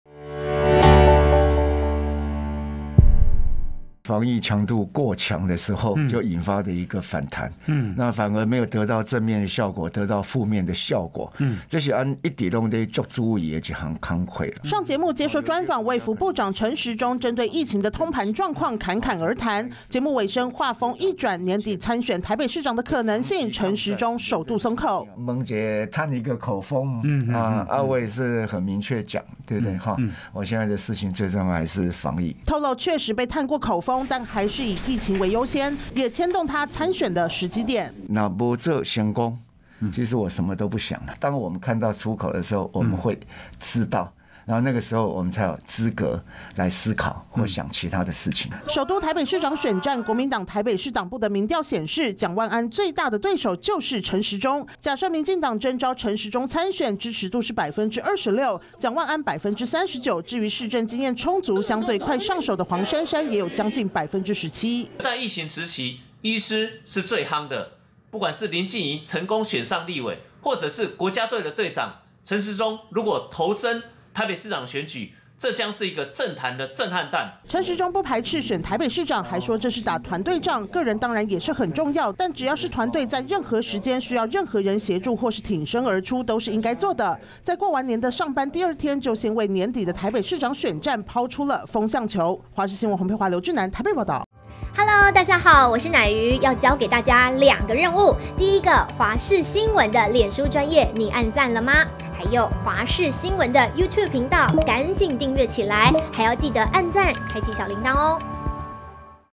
參戰台北市長選舉? 陳時中"不排斥"｜華視新聞 20220208
年底的台北市長選舉，衛生福利部長陳時中，被視為是民進黨的熱門人選，呼聲也很高；他昨(7)日接受專訪的時候首度鬆口，不排斥選舉，強調團隊在任何時間，需要誰挺身而出都是應該做的，但他不喜歡吃碗內看碗外，還是先把防疫做好，才會去思考其他的事情。